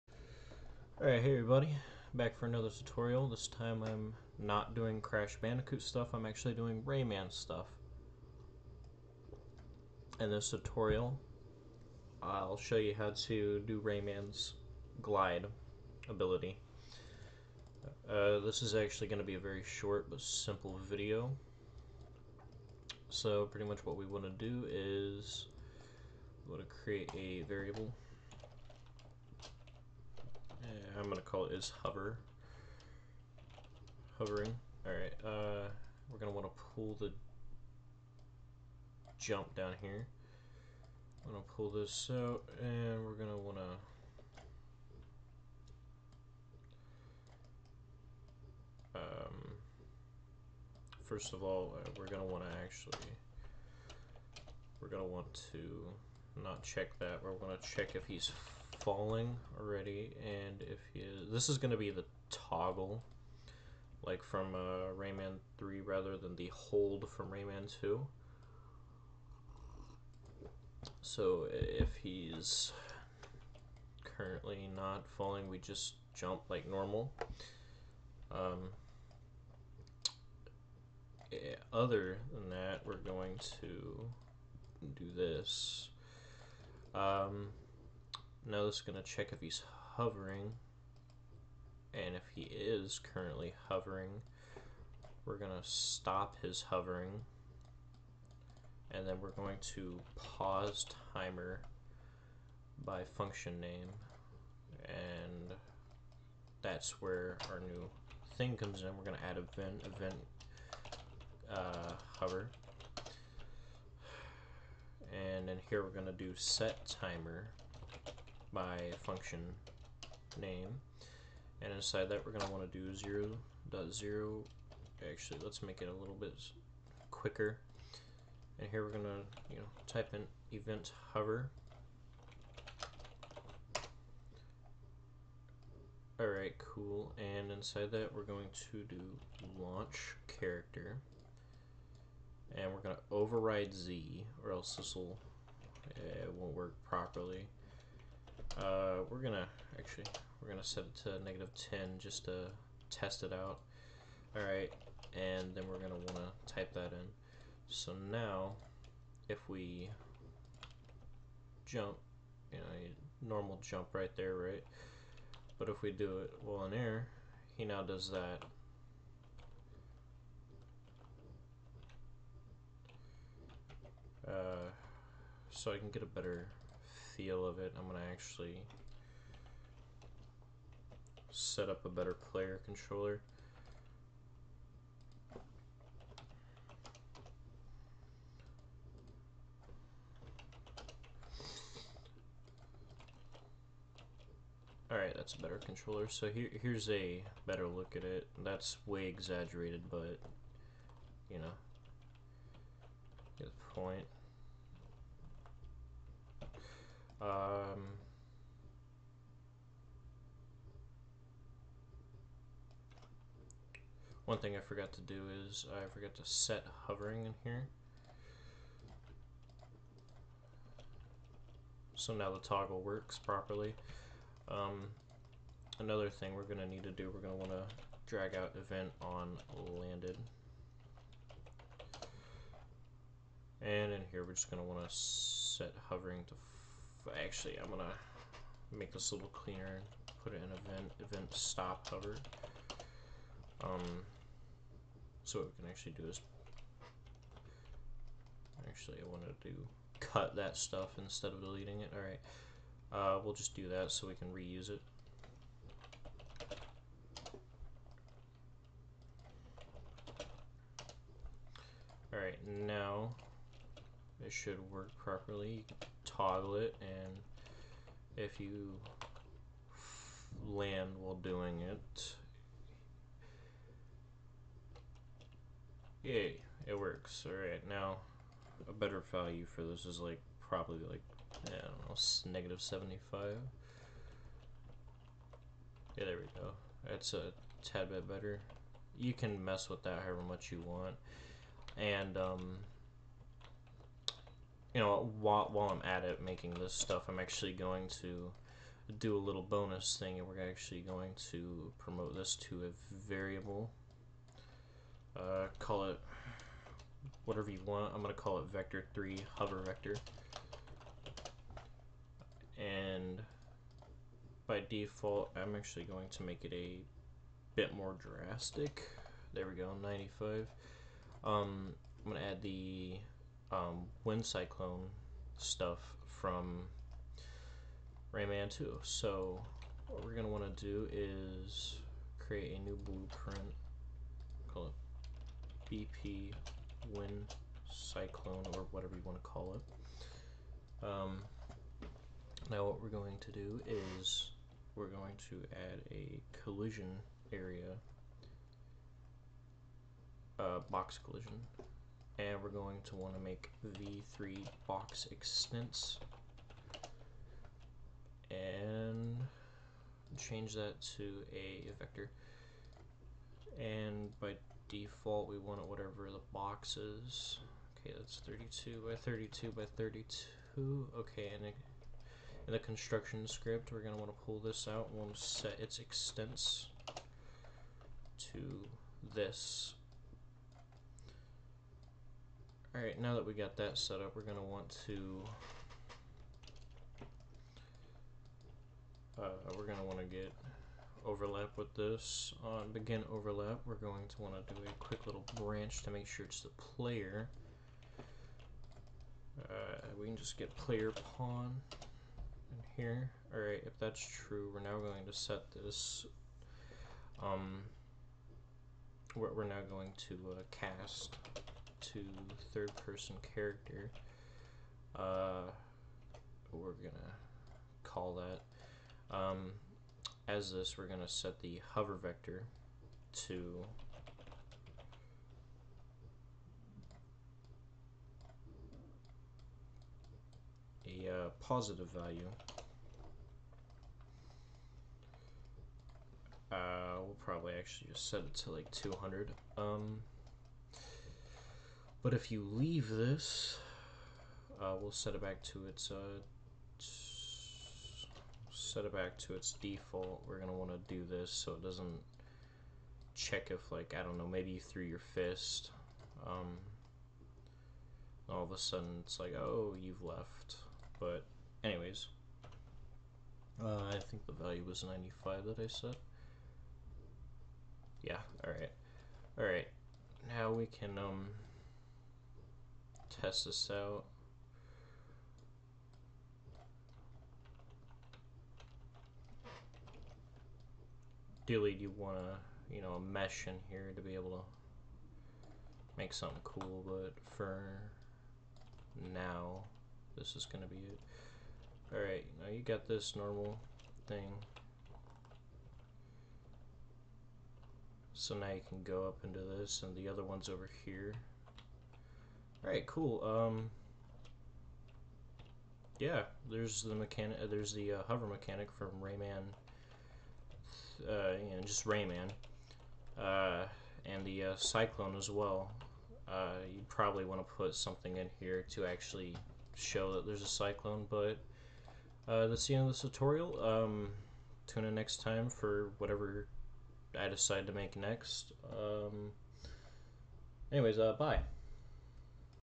[Unreal Engine 4 Tutorials] Rayman's Hover Ability in Blueprints
In this tutorial we attempt remaking Rayman's iconic hover ability.